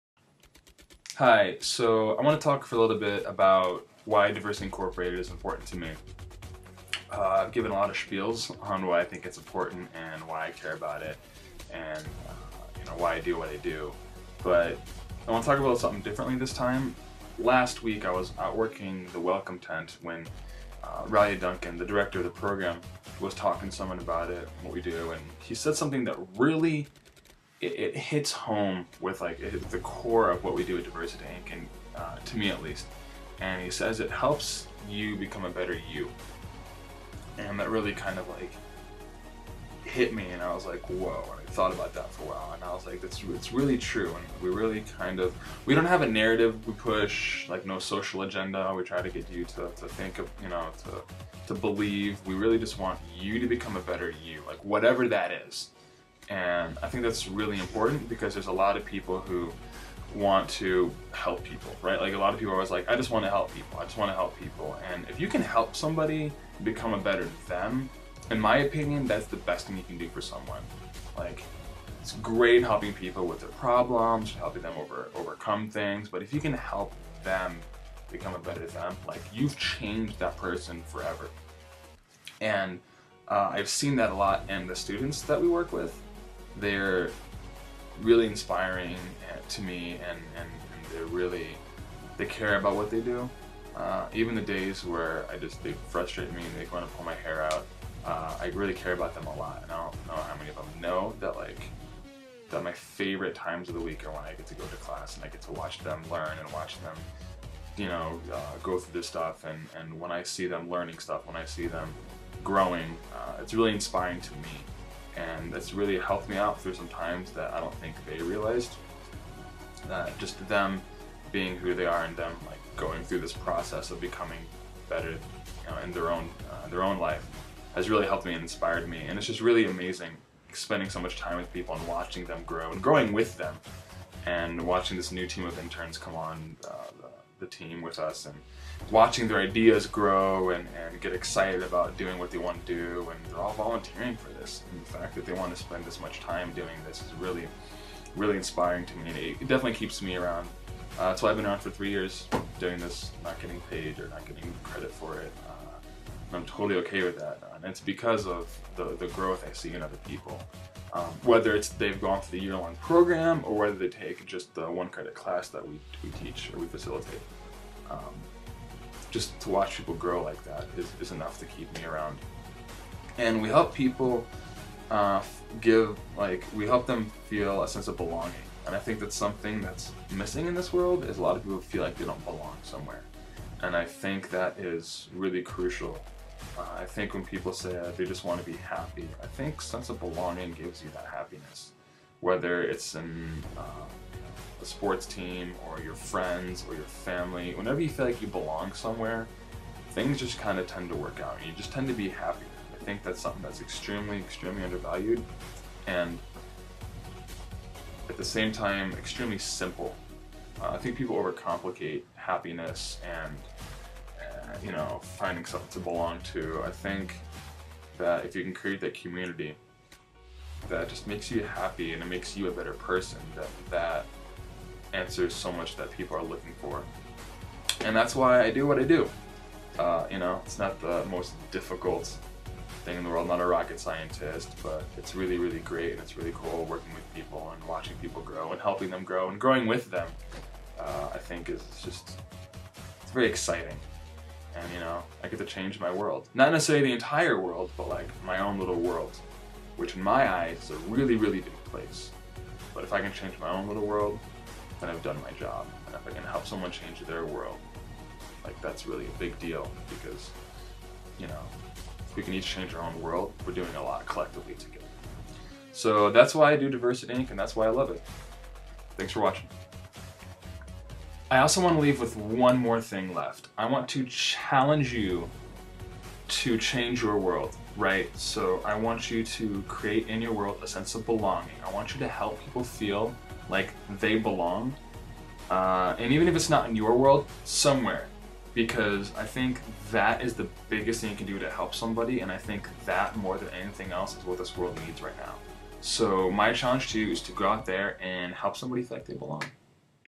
Why DiversityInc is important to me.
This is why I work so hard with DiversityInc and why it matters to me.